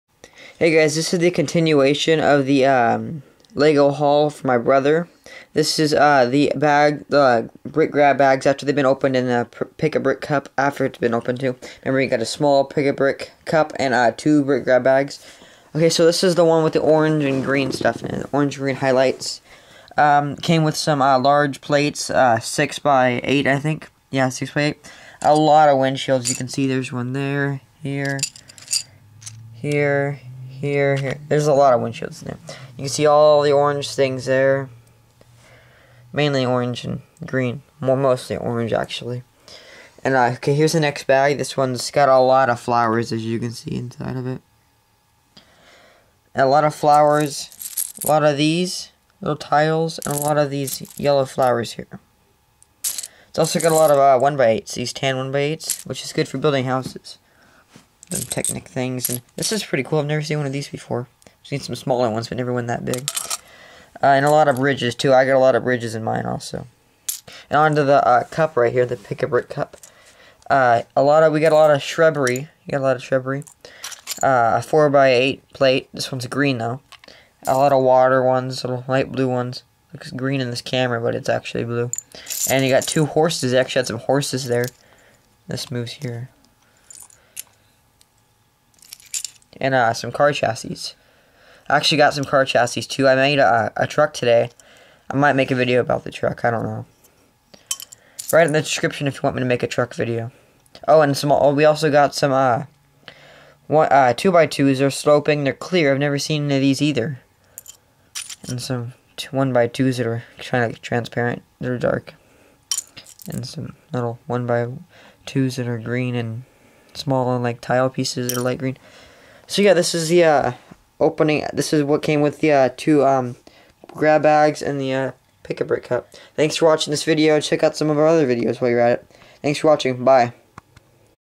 Continuation of Haul (1-28-12)
This is a continuation of my brothers Lego haul. (1-28-12)